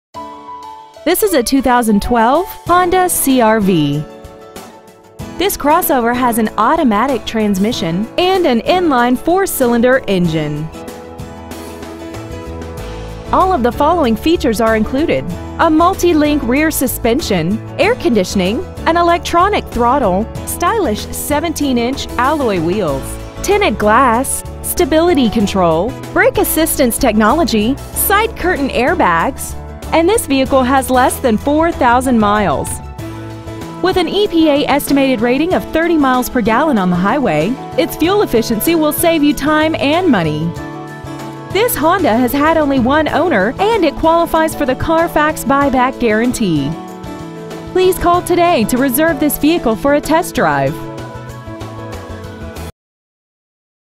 Preowned 2012 Honda CR-V Philadelphia PA 19116
We have been honored to serve the Philadelphia PA area , we promise that your experience at our dealership will exceed your expectations !

 Year : 2012
 Make : Honda
 Model : CR-V
 Engine : 2.4L DOHC MPFI 16-valve i-VTEC I4 engine
 Trans . : Automatic
 Exterior : Silver
 Miles : 3,757
 Interior : Gray